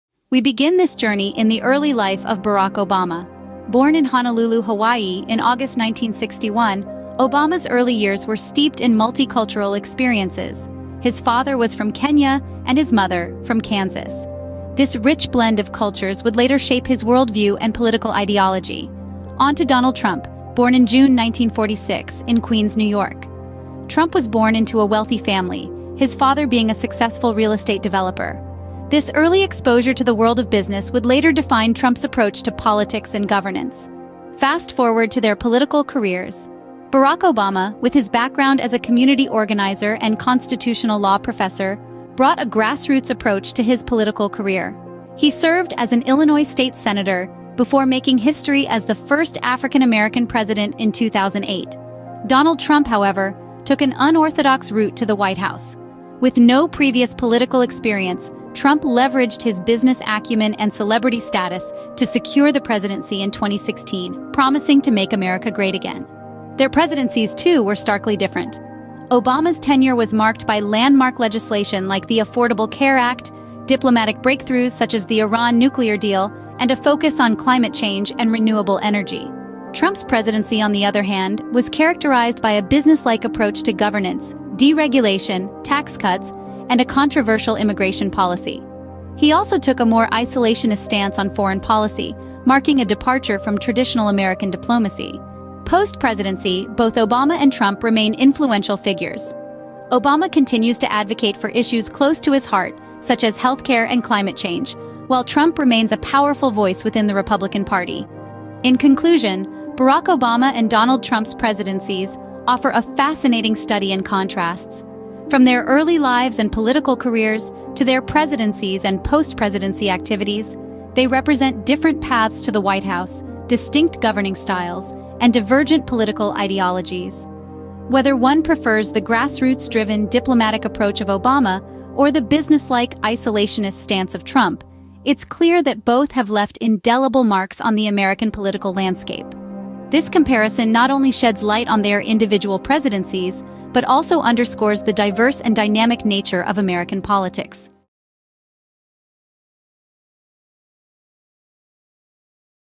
Barack Obama vs Donald Trump in 2 Minutes
A blow-by-blow look back at the first 100 days in office of Presidents Barack Obama and Donald Trump--plenty of time to get a good feel for what their entire presidencies would be like.